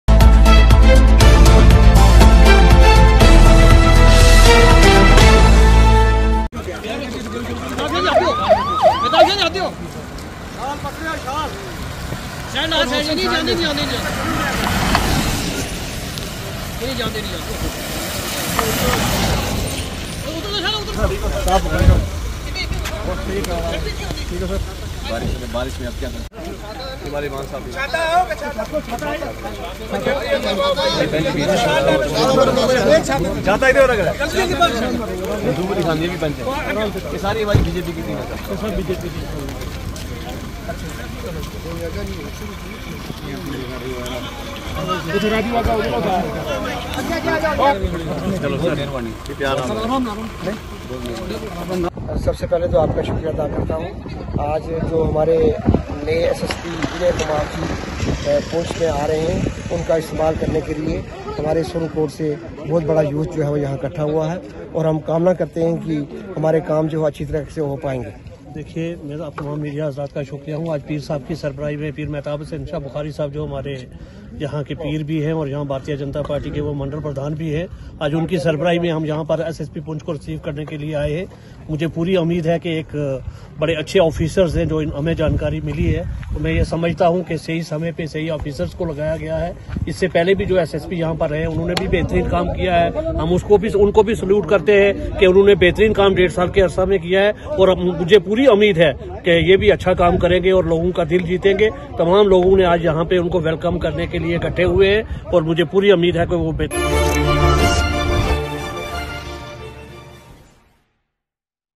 SSP Vinay kumar was Warmly Welcomed on Reaching Surankote
Poonch News..SSP  Vinay kumar was warmly welcomed on reaching Surankote